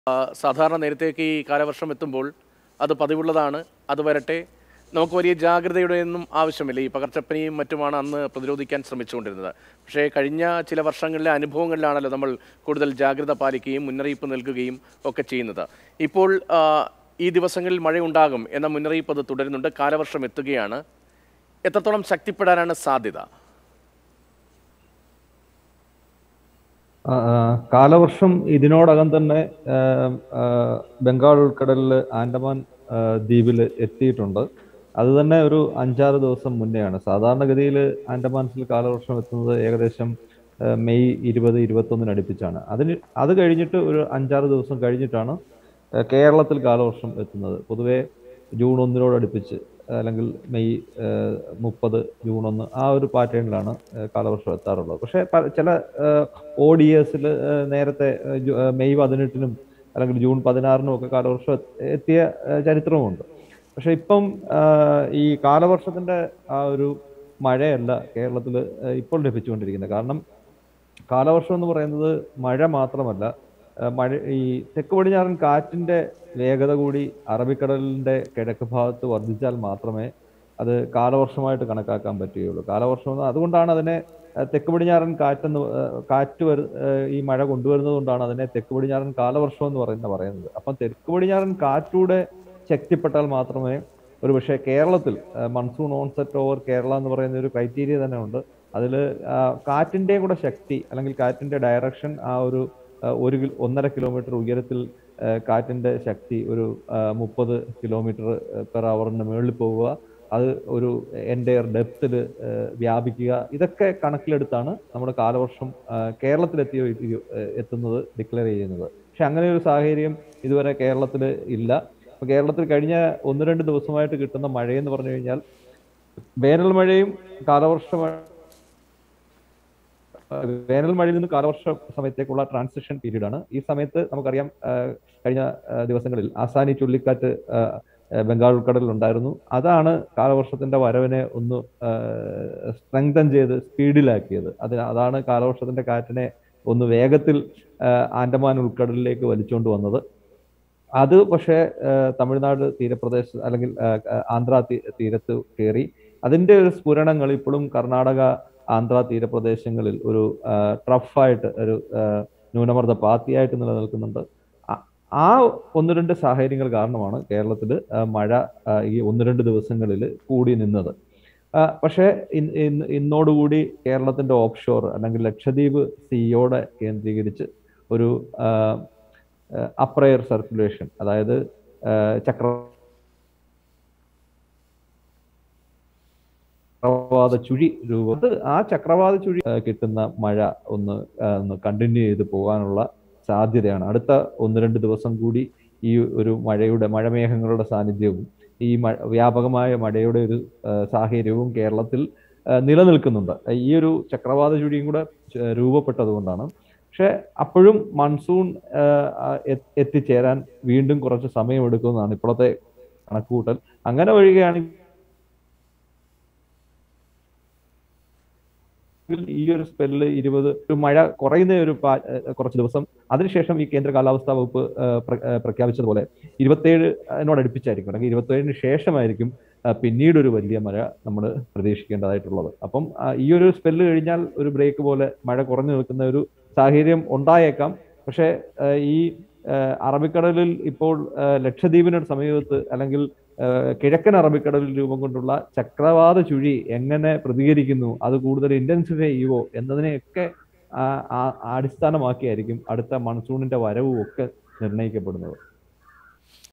ബം​​ഗാൾ ഉൾക്കടലിലുണ്ടായ ആസാനി ചുഴലിക്കാറ്റ് കാലവർഷത്തിന്റെ വരവിനെ ശക്തിപ്പെടുത്തി; ഡോ എസ് അഭിലാഷ്
കിഴക്കൻ അറബിക്കടലിൽ രൂപപ്പെട്ട ചക്രവാത ചുഴി എങ്ങനെ പ്രതികരിക്കുന്നു എന്നതിനനുസരിച്ചാണ് അടുത്ത മൺസൂൺ വരവ് നിർണ്ണയിക്കുന്നതെന്ന് ഡോ എസ് അഭിലാഷ് സ്പാര്‍ക്ക്@3 ചര്‍ച്ചയിൽ.

Happy viewing!